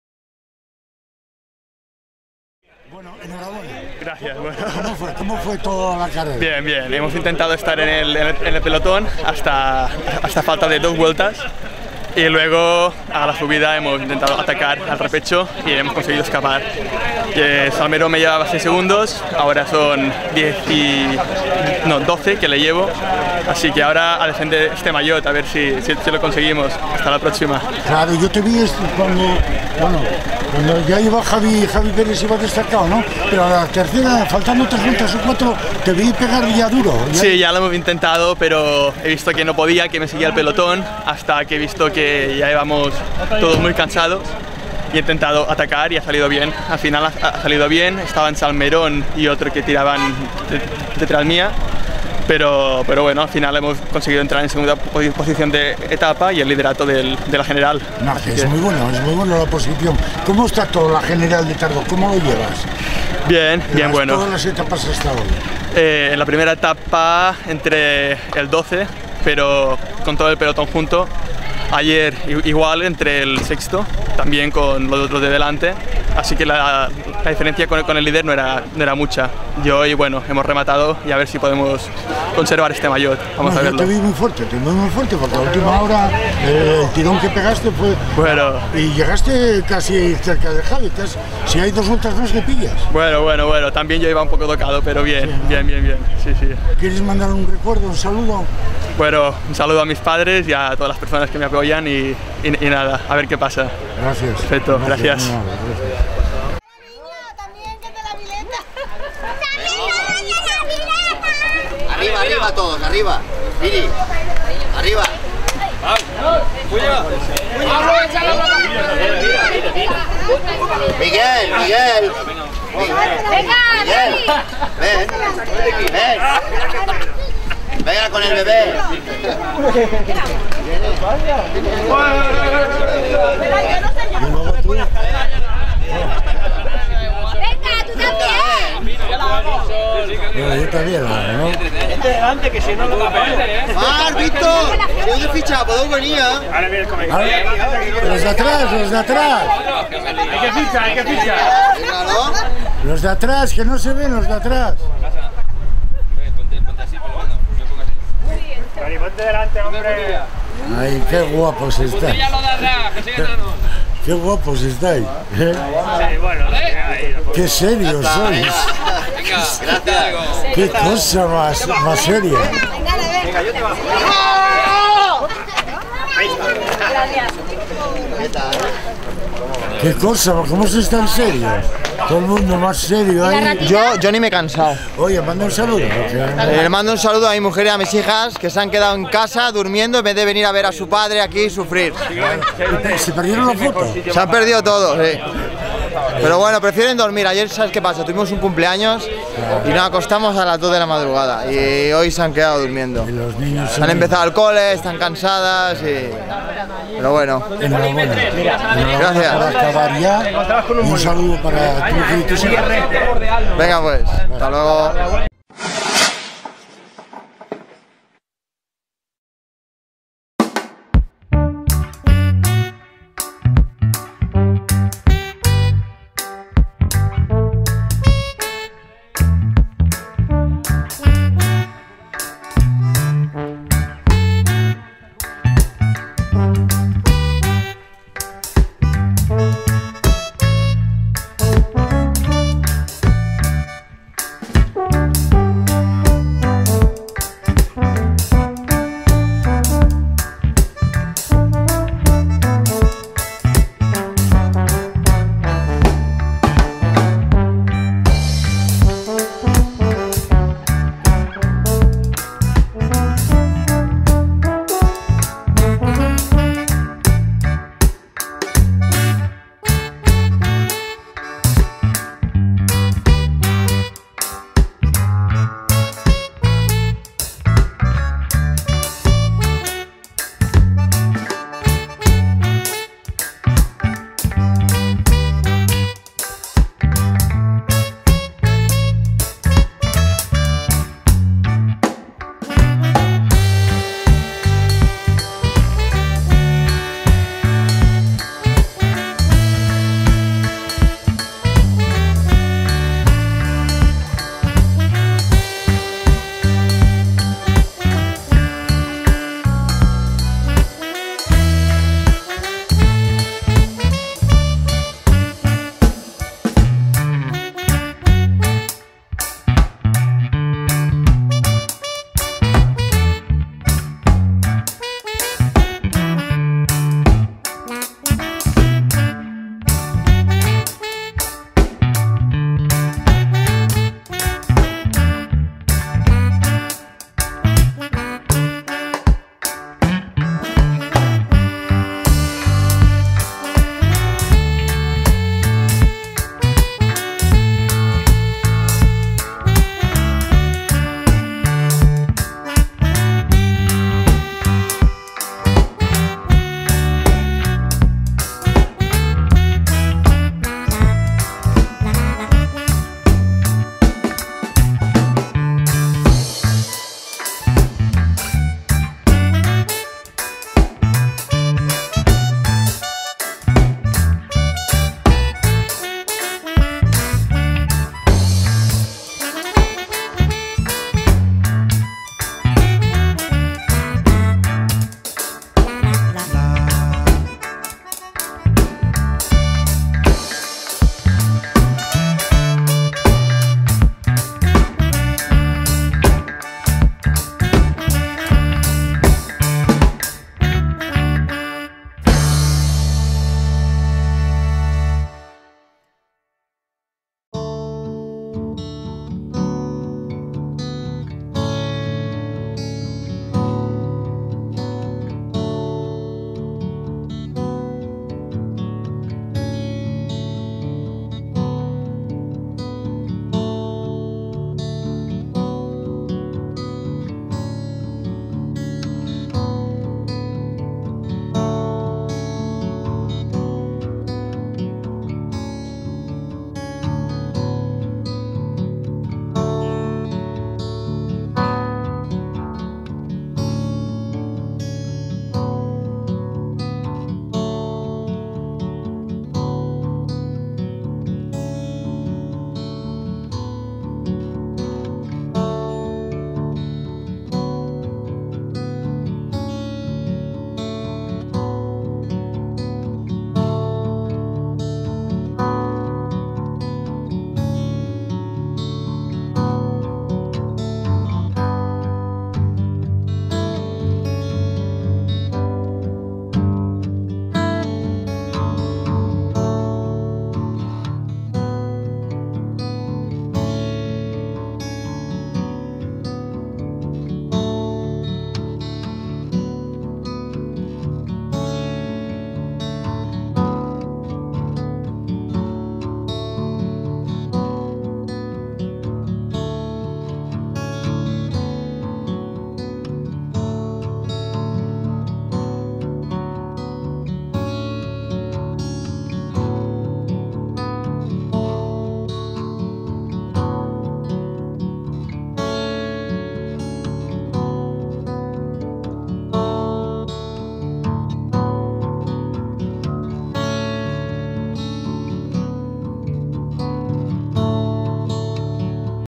Nº 1454 MIGUEL VALLS LUIS GARCÍA XV CHALLENGER TARDOR SANTA PONSA SANTA PONSA MALLORCA ELCAMARONIN
Descripción CHALLENGER TARDO SANTA PONSA CALVIÁ
Otra etapa más, dura donde las haya y después de ayer cobrar otra etapa nos encontramos con los grandes, únicos en esta selva de campeones, están todos los que son, los sin miedo, los que tienen dos cojones para hacer lo que haga falta y con una sonrisa.
Tenían que estar inscritos muchos más pero era lo que había y el campeón de baleares Javi Peris, lo sabia, Miguel Valls lo sabia, Jaume Font, lo sabia, Juan Molinero también los sabia Coco Sineu lo sabia y lo intuía, Toni Balleter, Alex Martinez el equipo de VIwo que ninguno de sus corredores se queda atrás.Pero sabian que la prueba era muy dura y que nadie regala nada, solo el esfuerzo les valía, me alegra ver que no les salen las cosas pero que siguen, son campeones todos ellos y merecen un gran y único respeto y admiración como yo les tengo.
Prueba con mucho viento, pero ellos me dicen que lo han pasado bien, pero yo digo que hay que pedalear cuesta abajo también, felicidades a todos, organización, Policía municipal, ambulancia, a todos los que han participada de una o otra forma.
Jóse ELCAMARONIN